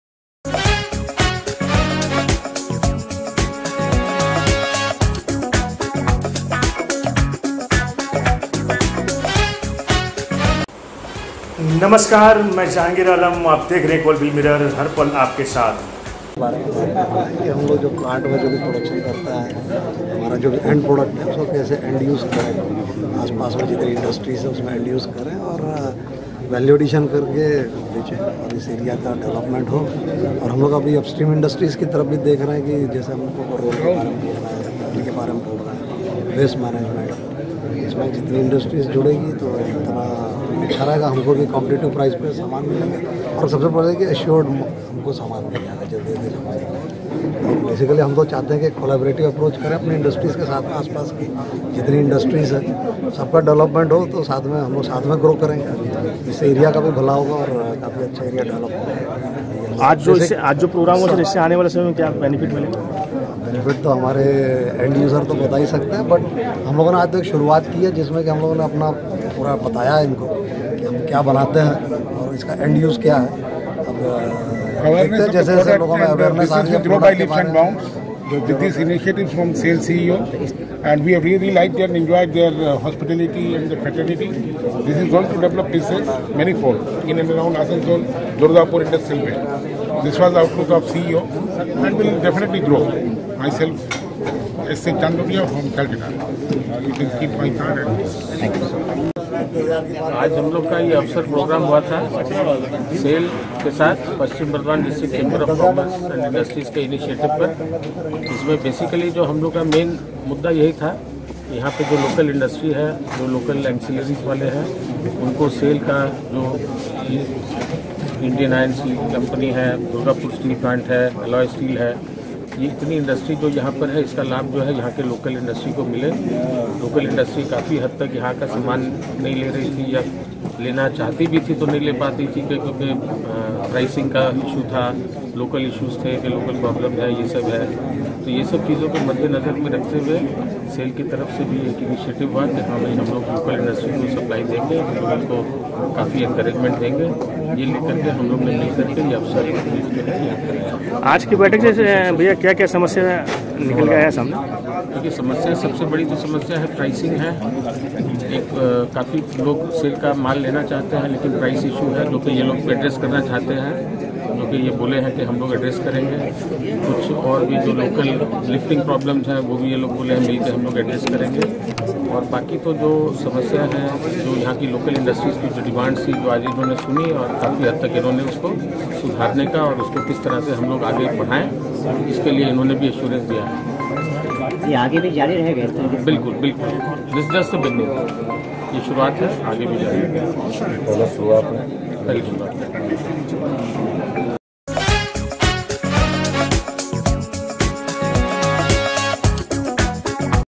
CoalField MirroR : आईएसपी, डीएसपी, एएसपी के साथ पारस्परिक सत्र का आयोजन : 09.11.19
पश्चिम बंगाल : स्टील औथोरीटी ऑफ इंडिया लिमिटेड (सेल) और पश्चिम बर्दवान जिला चेंबर ऑफ कार्मस के संयुक्त प्रयास से शुक्रवार को बर्नपुर क्लब में आईएसपी, डीएसपी, एएसपी के साथ पारस्परिक सत्र का आयोजन किया गया। इस दौरान जिले के व्यवसाईयों ने अपनी व्यावसायिक समस्याओ को आईएसपी-डीएसपी के अधिकारियों संग साझा किया। सीईओ ए.वी.कमलाकर ने व्यवसाइयों के प्रश्नो का उत्तर दिया और उनकी बातों को बारीकी से सुना एवं समाधान का आश्वासन दिया। मौके पर उद्दोगपति पवन गुटगुटिया, वीके ढल सहित शिल्पांचल के गणमान्य उद्दोगपति उपस्थित थे।
West Bengal: A joint session of the Steel Authority of India Limited (SAIL) and the West Burdwan District Chamber of Commerce organized an interactive session with the ISP, DSP, ASP at the Burnpur Club on Friday. During this period, the businessmen of the district shared their business problems with the officials of ISP-DSP. CEO AV Kamalkar answered the queries of the businessmen and listened closely to them and assured them the solution. On the occasion, industrialist Pawan Gutgutia, VK Dhal and other dignitaries from Shilpanchal were present.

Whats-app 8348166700